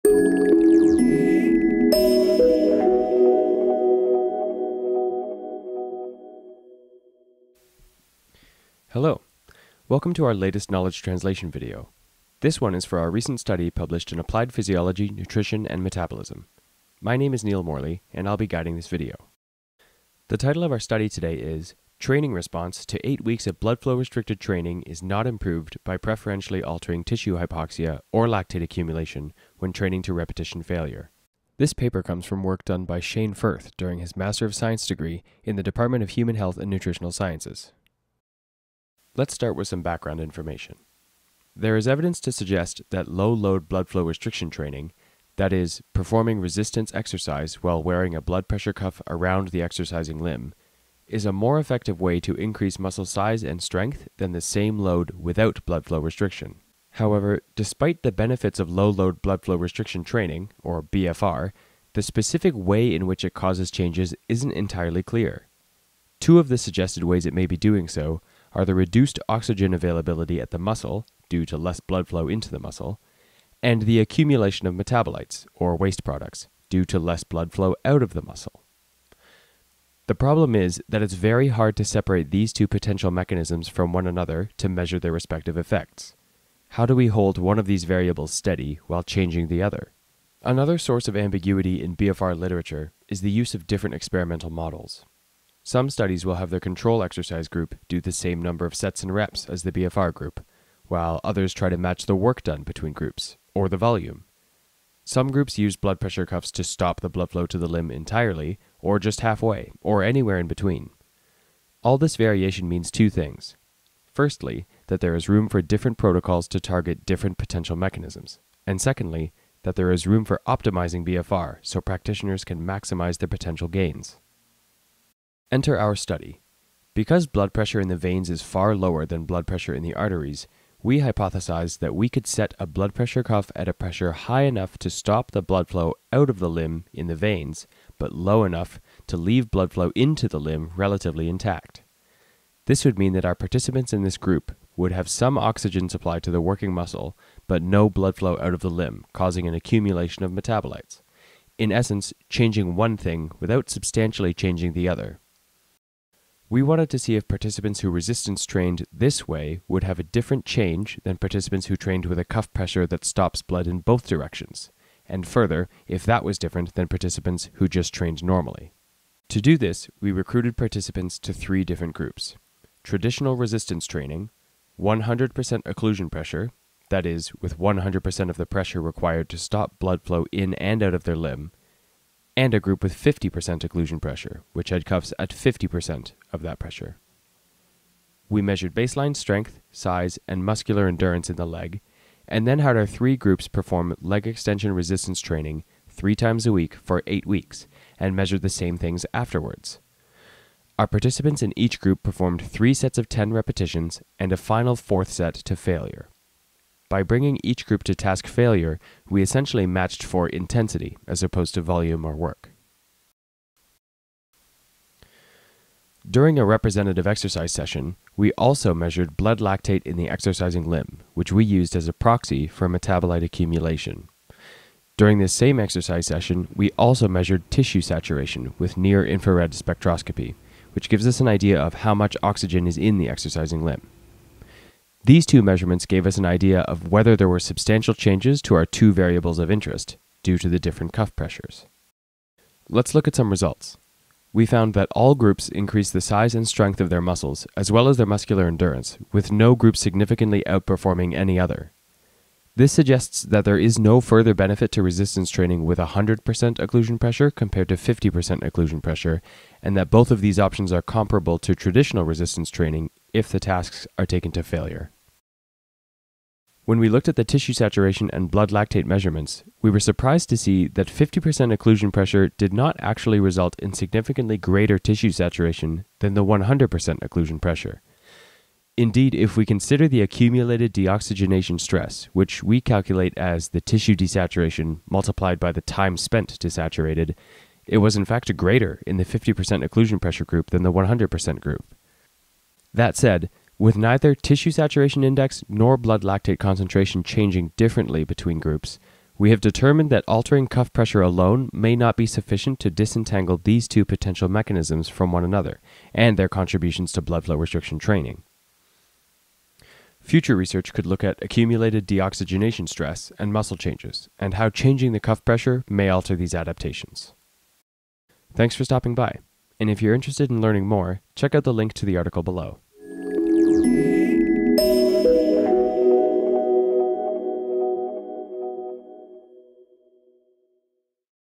Knowledge Translation: Blood Flow Restriction Training, Deoxygenation, and Metabolite Accumulation
Neil Morley walks us through the findings of our recent article published in Applied Physiology, Nutrition, and Metabolism.